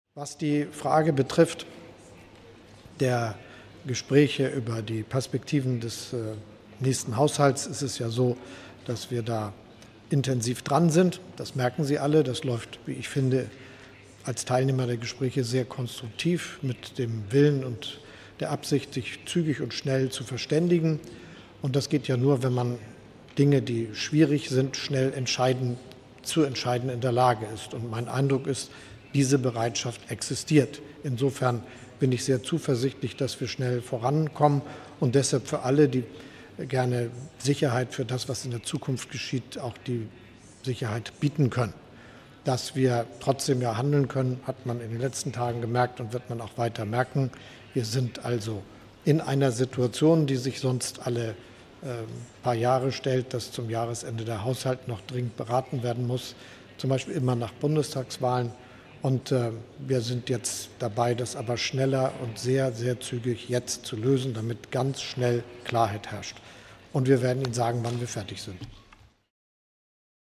Haushaltsstreit: Scholz erwartet "schnell Klarheit" | AFP
Bundeskanzler Olaf Scholz (SPD) hat sich zuversichtlich gezeigt, dass es im Streit um den Haushalt 2024 in Kürze eine Einigung geben wird. „Wir sind jetzt dabei, das schnell und sehr zügig zu lösen, damit ganz schnell Klarheit herrscht“, sagte Scholz in Berlin.